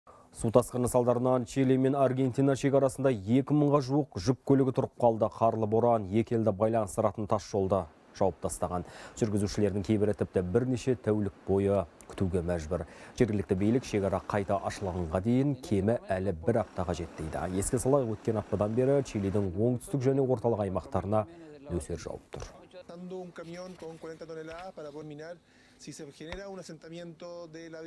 Су тасқыны салдарынан Чили мен Аргентина шекарасында кептеліс
Су тасқыны салдарынан Чили мен Аргентина шекарасында 2 мыңға жуық жүк көлігі тұрып қалды.